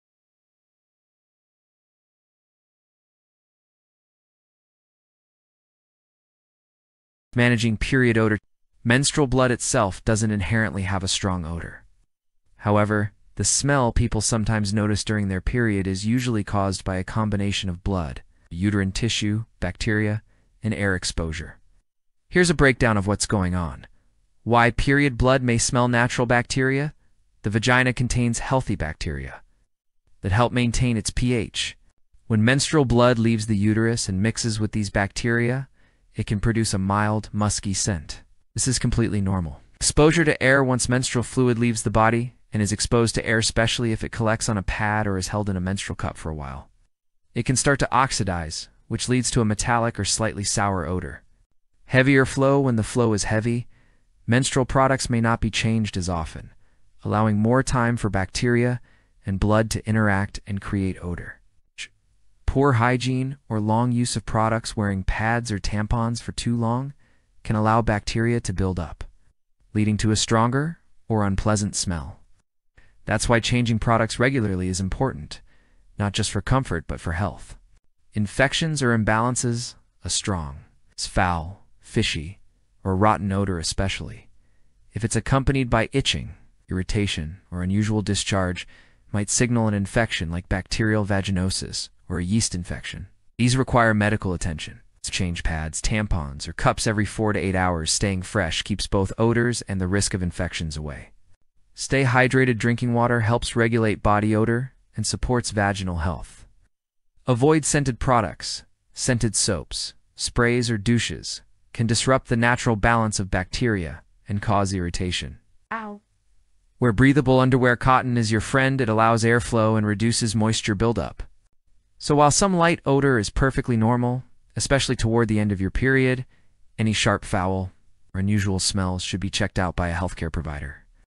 Why do periods smell?The real reason No one talks about
Why Do Menses Smell? | The Science Behind Period Odor

Ever wondered why menstrual blood has a distinct smell? In this video, we break down the science behind period odor—what causes it, how it changes, and whether it's normal. From the role of bacteria to the impact of hygiene and diet, we explore the factors that influence menstrual scent and share tips for managing it.

Watch now to learn more about your body and embrace period health with confidence!